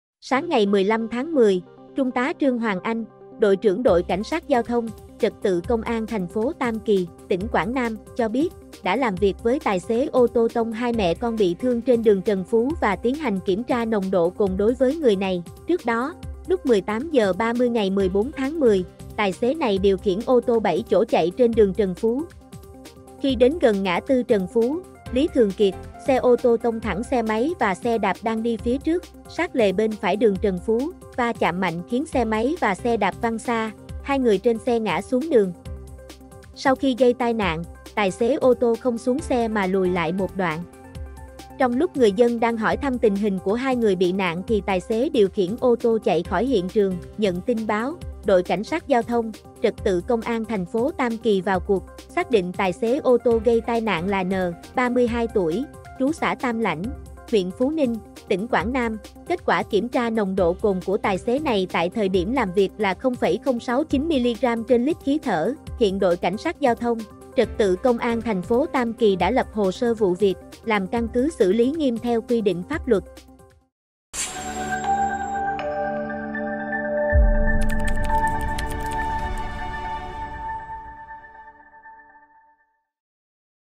Vụ tông 2 mẹ con ở Quảng Nam rồi bỏ chạy: Tài xế vi phạm nồng độ cồn
Sáng 15-10, Trung tá Trương Hoàng Anh, Đội trưởng Đội CSGT - Trật tự Công an TP Tam Kỳ (tỉnh Quảng Nam) cho biết, đã làm việc với tài xế ô tô tông hai mẹ con bị thương trên đường Trần Phú và tiến hành kiểm tra nồng độ cồn đối với người